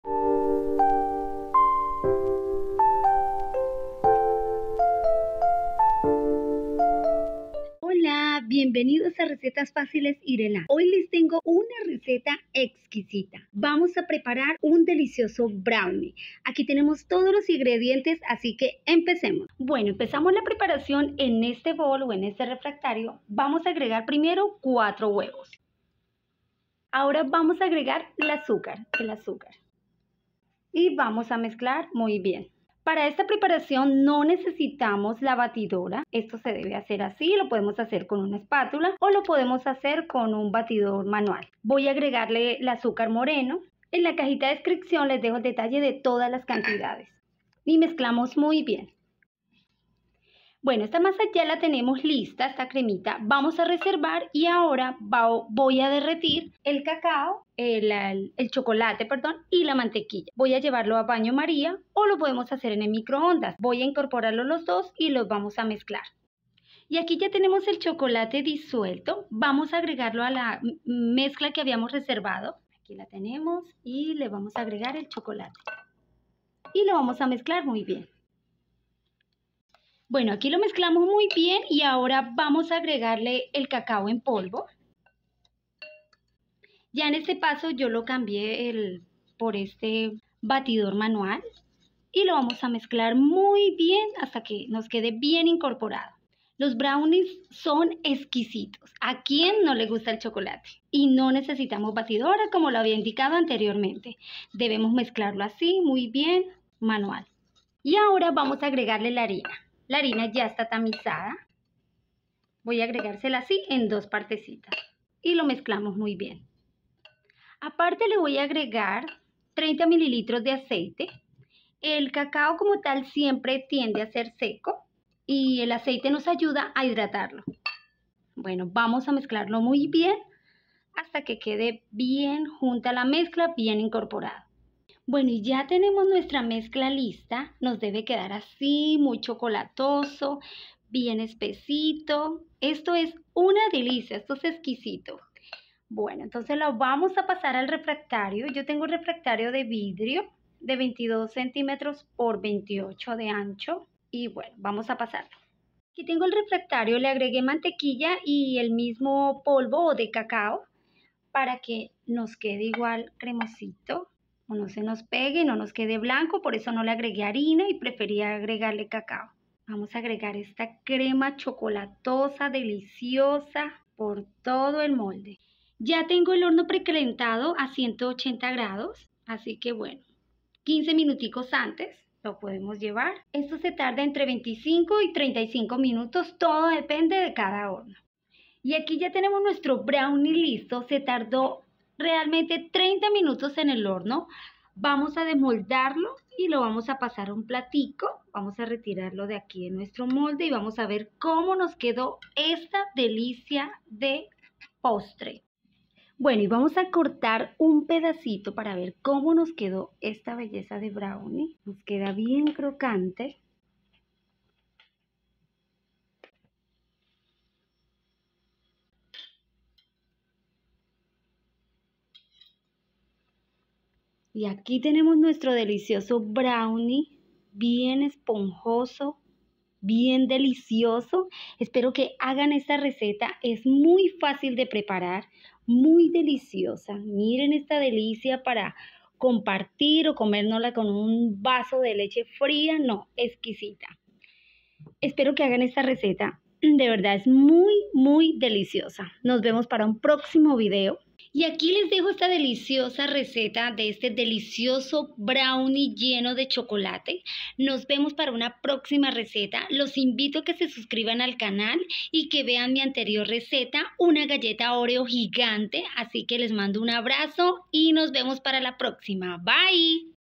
Delicioso Brownie con un Sabor Chocolateado
INGREDIENTES

• 4 huevos
• 120 g de azúcar
• 90 g de azúcar moreno
• 100 g de chocolate amargo
• 3 cucharadas de mantequilla
• 150 g de harina de trigo
• 1 pizca de sal
• 30 ml de aceite

PREPARACIÓN

1. Precalienta el horno a 180ºC, dejándolo calentar durante 15 minutos antes de usarlo.

2. Engrasa un refractario de vidrio y espolvorea cacao en polvo para sustituir la harina y evitar que el brownie salga blanco.

3. En un recipiente, bate los huevos y el azúcar hasta obtener una crema. Luego, agrega el azúcar moreno y mezcla bien. Reserva esta mezcla.

4. Derrite la mantequilla y el chocolate en el microondas o a baño maría. Cuando estén completamente derretidos, agrégalos a la crema reservada y mezcla bien.

5. Continúa añadiendo la harina, el cacao en polvo, el aceite y la pizca de sal a la mezcla. Incorpora todos los ingredientes de manera homogénea.

6. Vierte toda la mezcla en el molde que llevarás al horno, el cual ya debería estar precalentado. El tiempo de cocción varía, pero generalmente tomará entre 25 a 35 minutos. Para verificar si está listo, introduce un palillo; si sale un poco húmedo, significa que está listo.

7. Retira el brownie del horno, córtalo en cuadritos y ¡disfruta de esta deliciosa golosina de brownie!